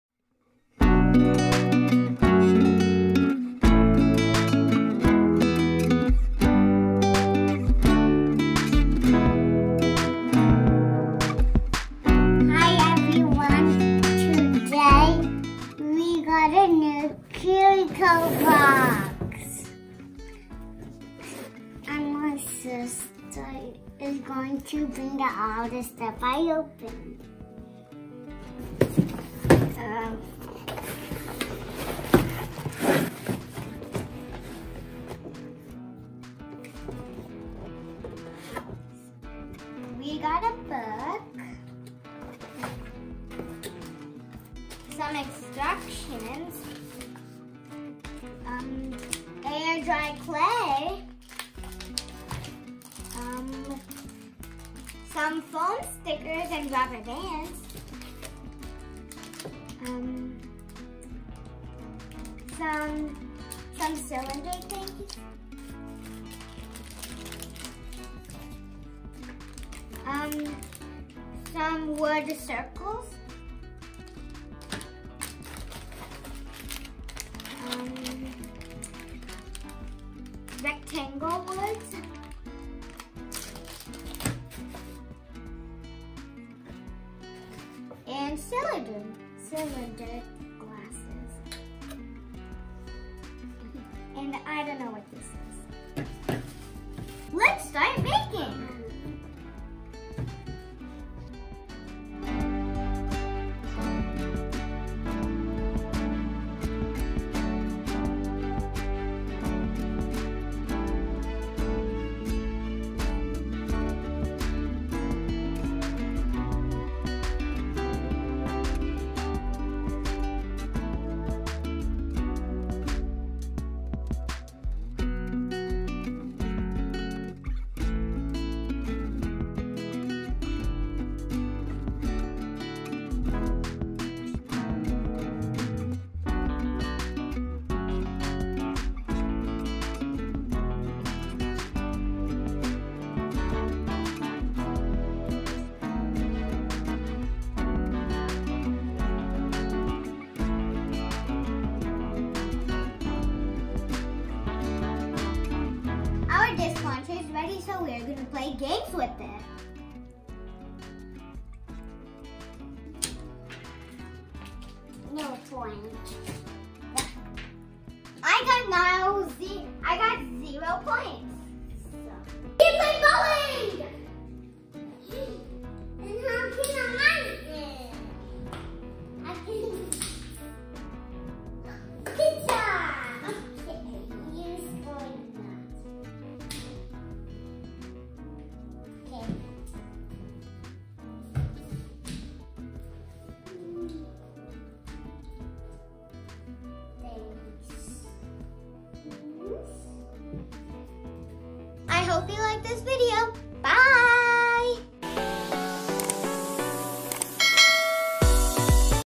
Disk Launcher - STEM- Kiwico - Kiwi Crate - 2020
Fun with Education!

Watch how to create Disk Launcher!